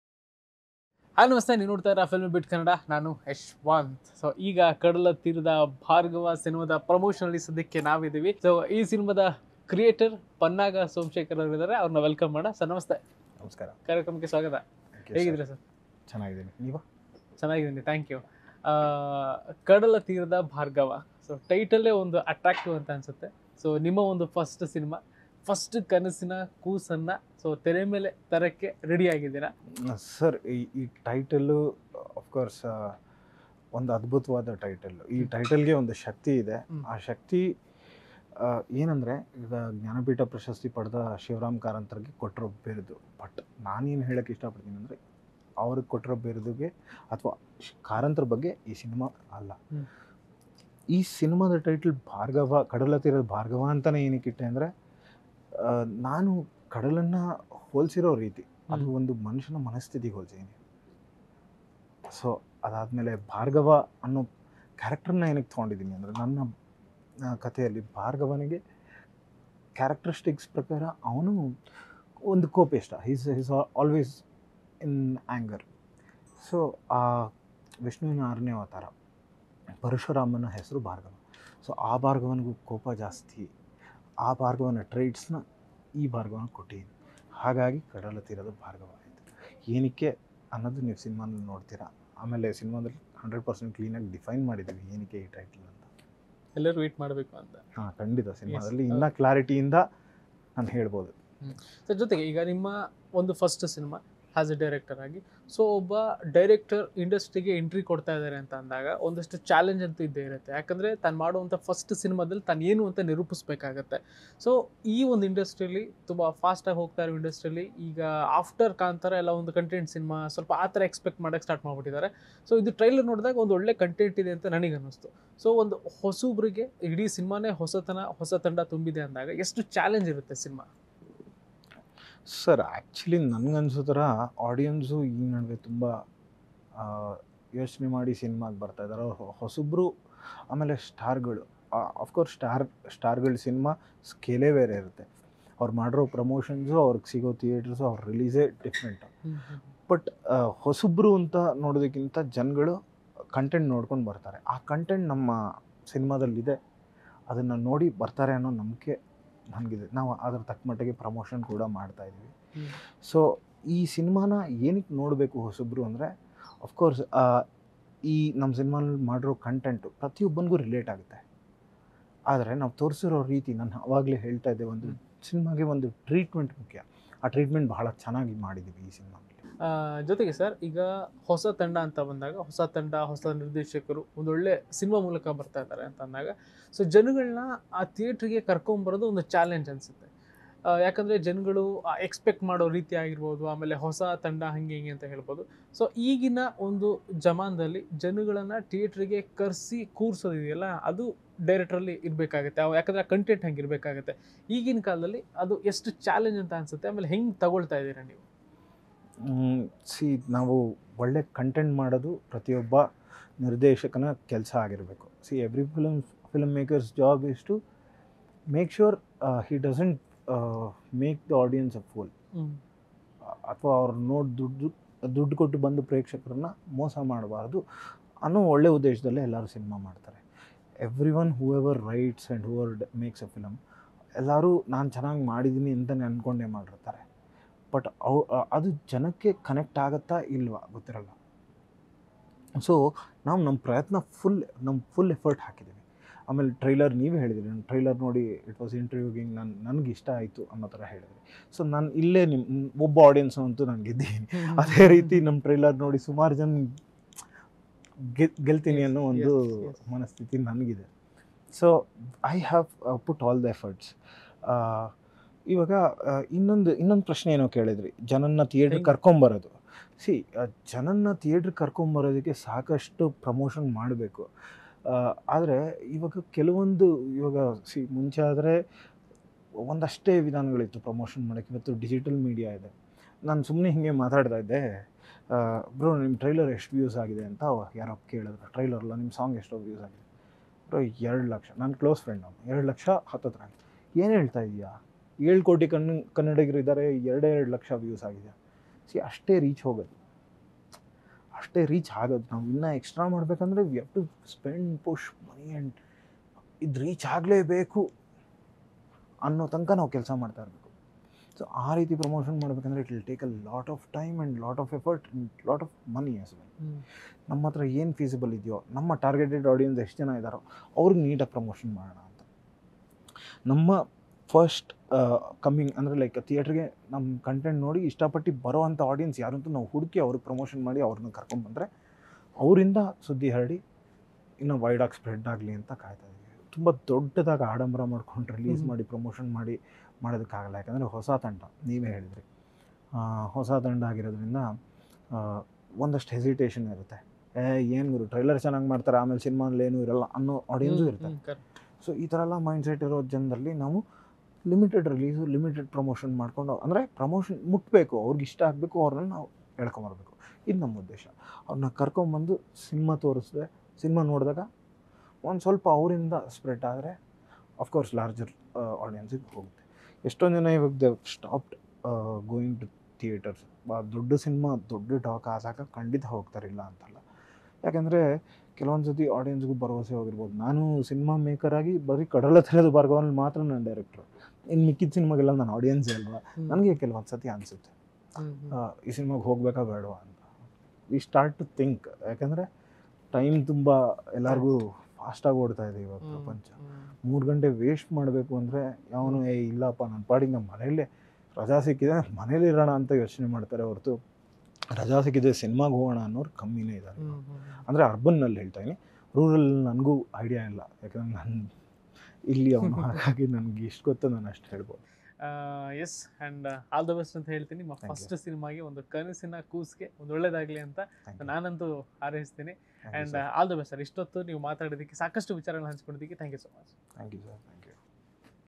Kadala Theerada Bhargava ಟೈಟಲ್ಲೇ ಒಂದು ಶಕ್ತಿ | Pannaga Somashekar | Filmibeat
ಕಡಲ ತೀರದ ಭಾರ್ಗವ ಸಿನಿಮಾದ ಮೂಲಕ ಎಂಟ್ರಿ ಆಗುತ್ತಿರುವ ಯುವ ನಟ ವರುಣ್ ಅವರ ಮಾತು...

This is the words of young actor Varun, who is making his entry through the movie Kadala Theerada Bhargava